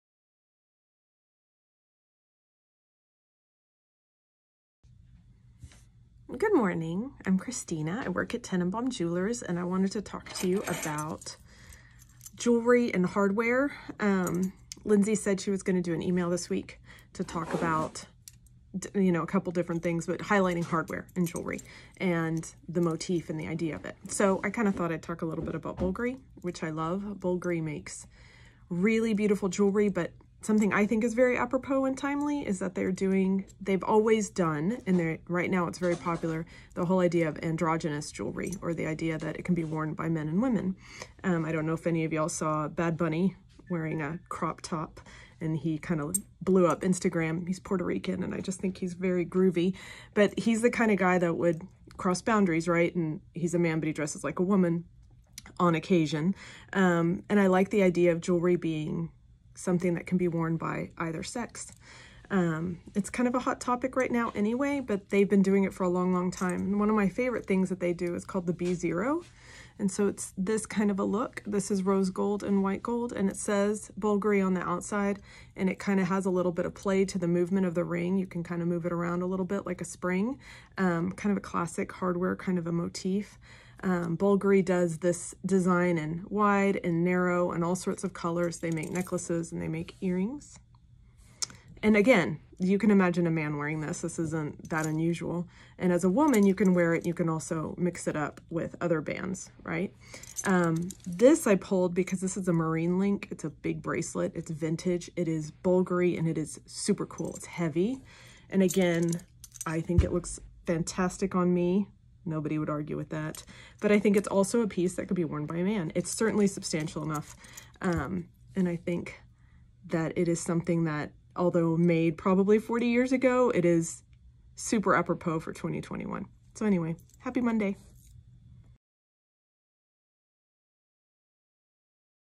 Bulgari: Gender Neutral Hardware Jewelry
Learn about some of Bulgari's classic gender-neutral jewelry designs!